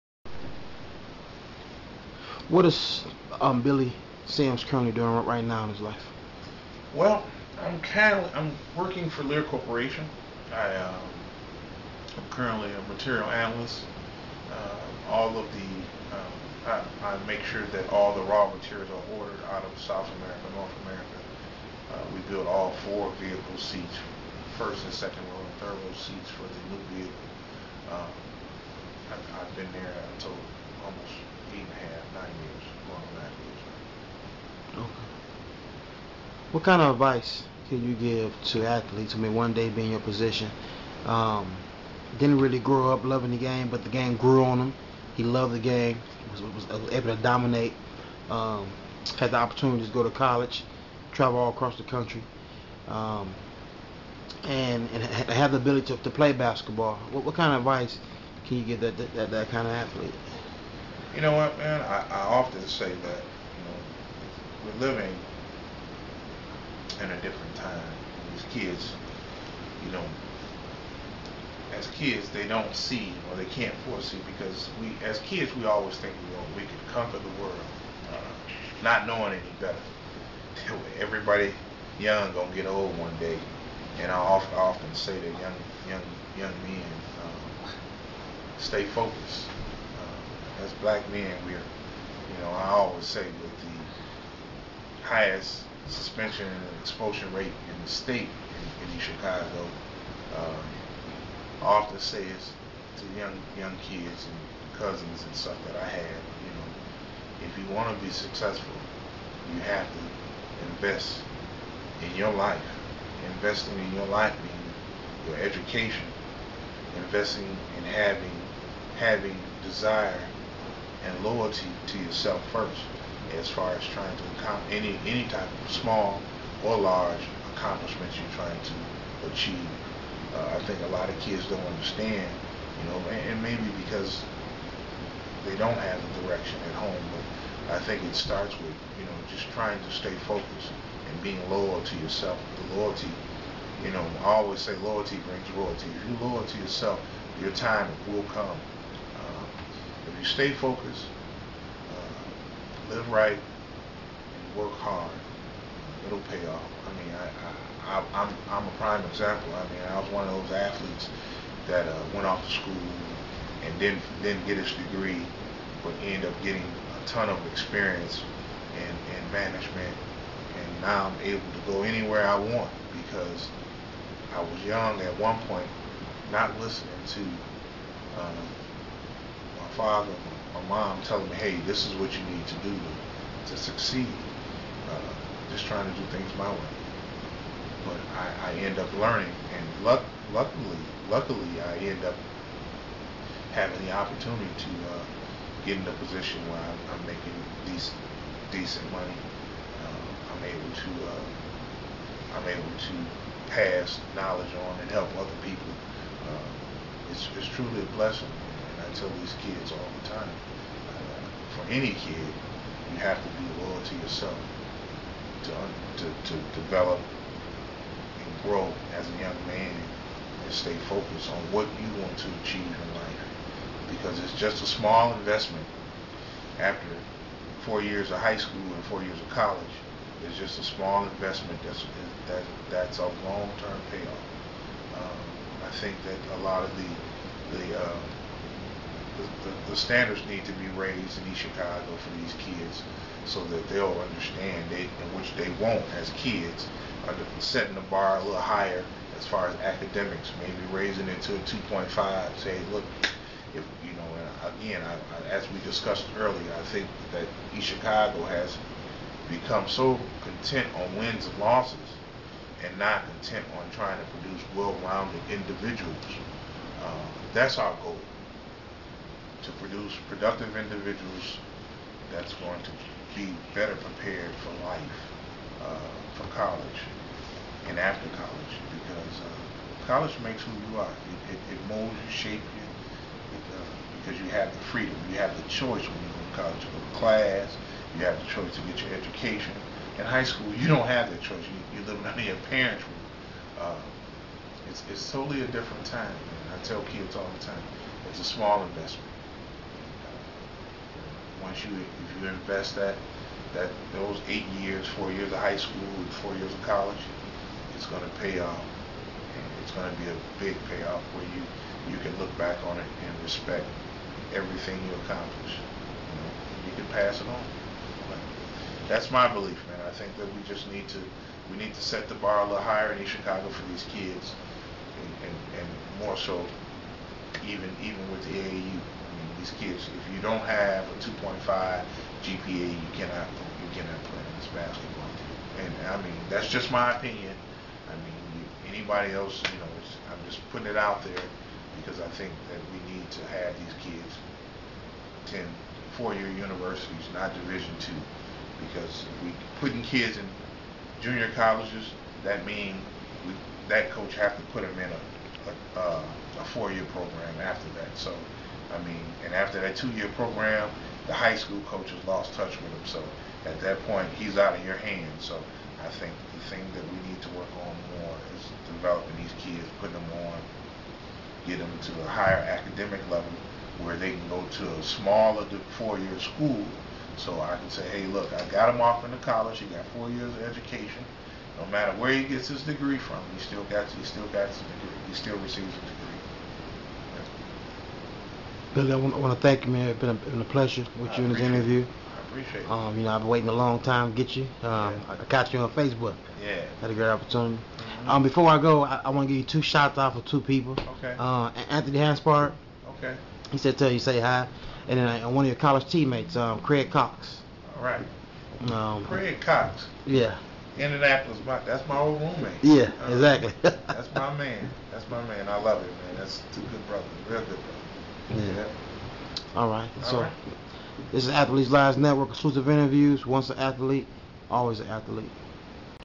Part 6 of ALN exclusive interview with Billy Sims
with Billy Sims, star basketball player at EC Roosevelt class of 86. Billy talks about growing up in East Chicago, early influences in sports, playing days in high school and college, and who he considers the best basketball in ECC history!